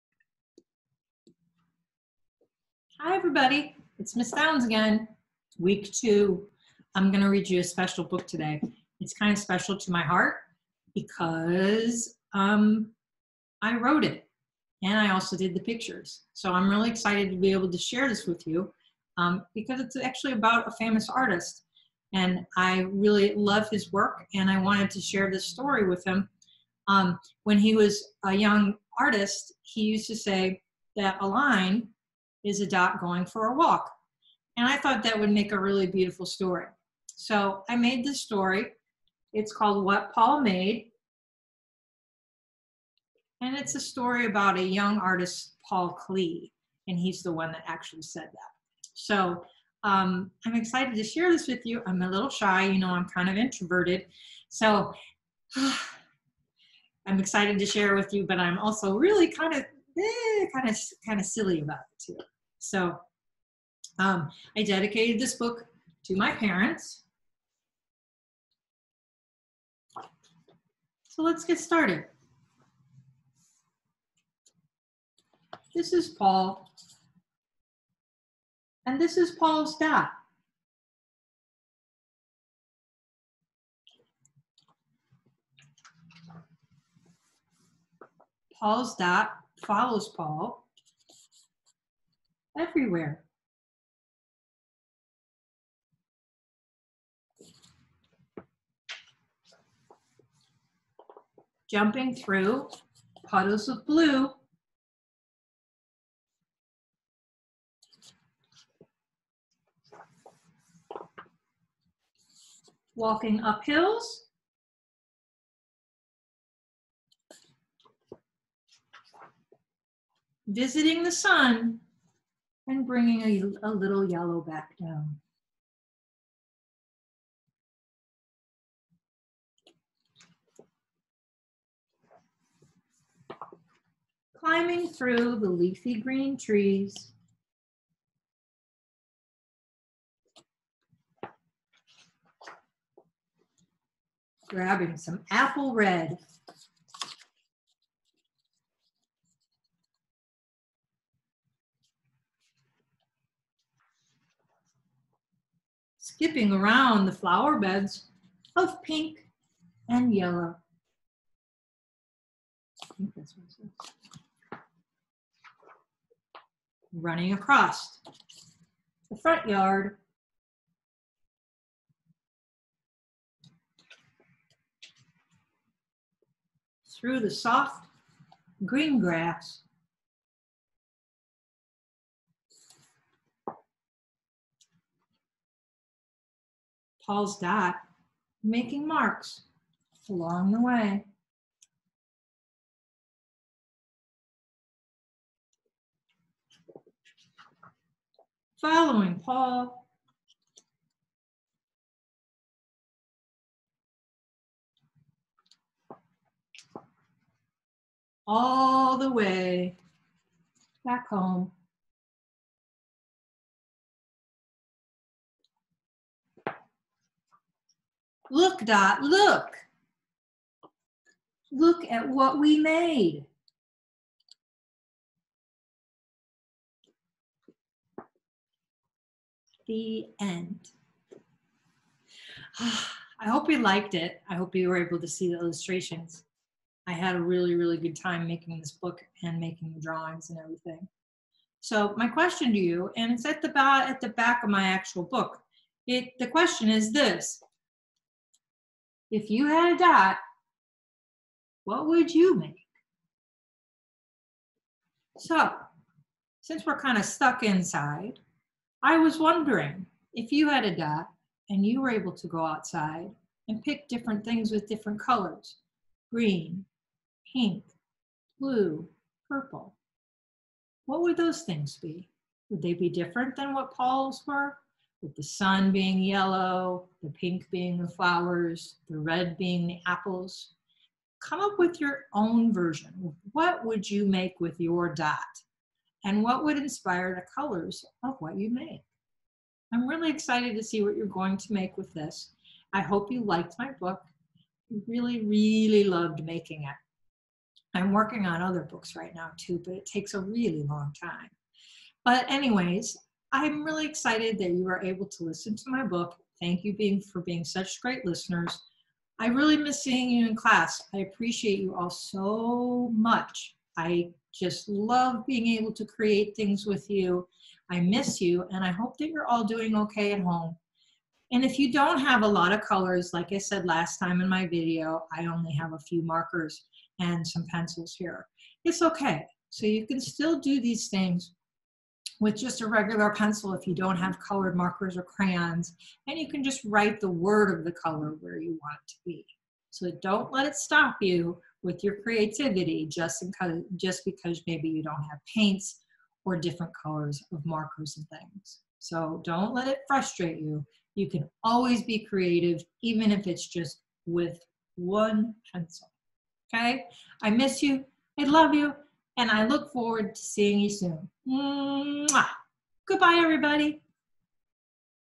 PS - 1st : What Paul Made - A story about a young Paul Klee with art prompt.
A story based on Artist Paul Klee's quote " A line is a dot going for a walk." Ms. Downs reads her own children's book leading students into a creative adventure of their own. Minimal supplies needed.